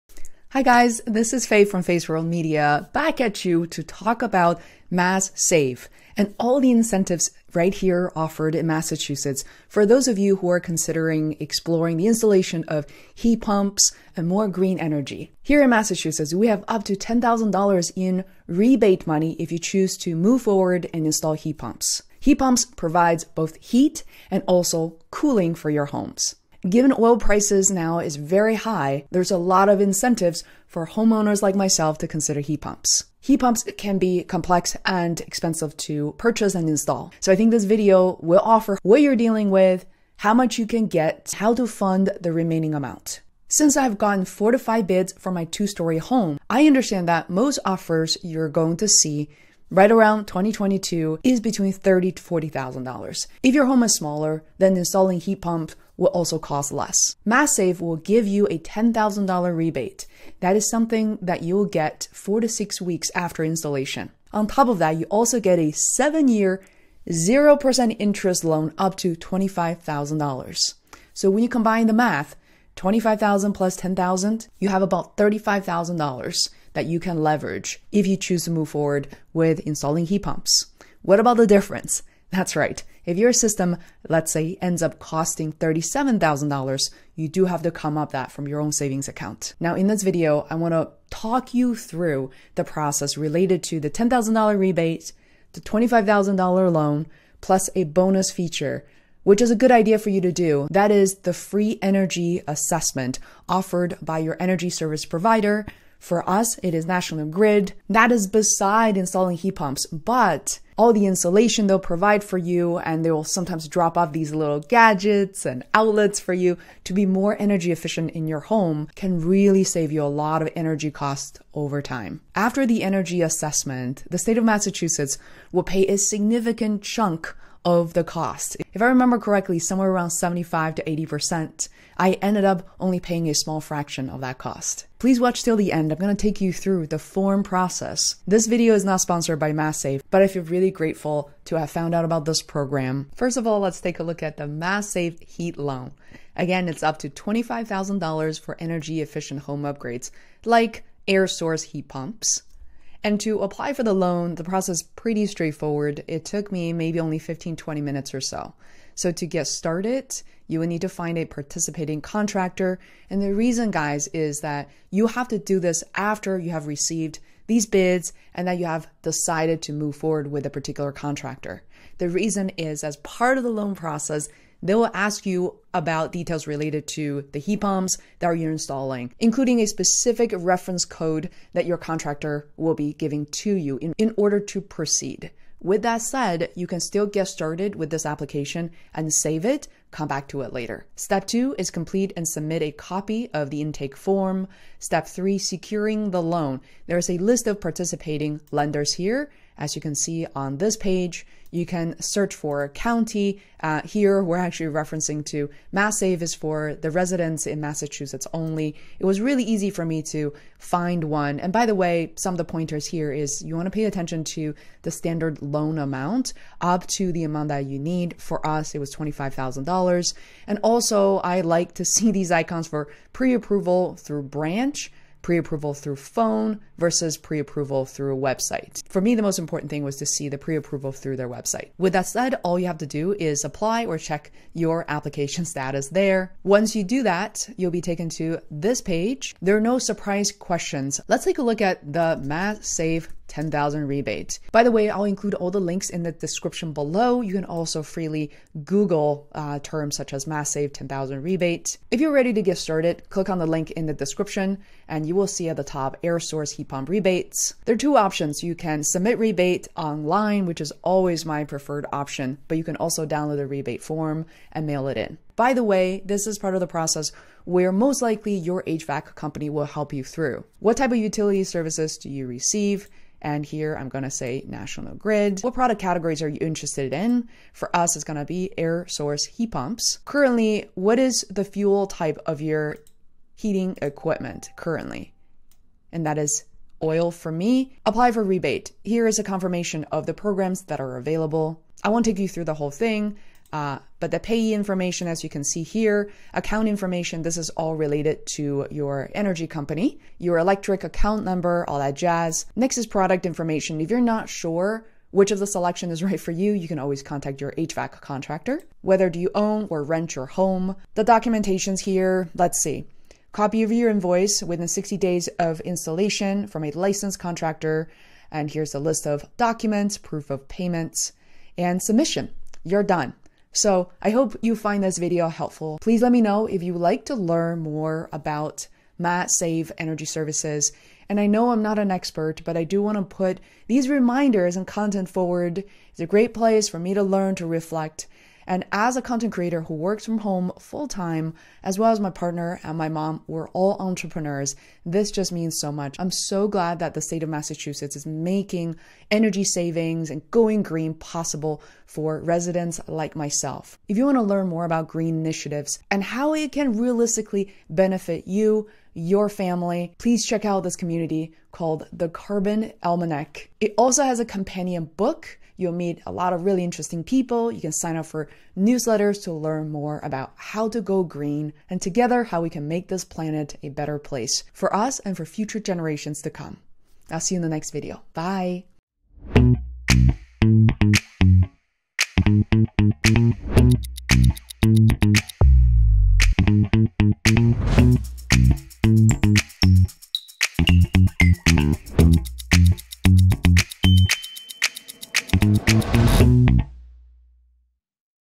MassSave 10K HEAT rebates PLUS 0% interest loan up to 25K (with forms walkthrough!)
In this video I talk about the Mass Save incentives offered in Massachusetts for those of you who are considering the installation of heat pumps and using green energy. I will also walk you through the form to apply for the 10k rebates.

🍨 Support Feisworld by buying me some ice cream (thank you! xo)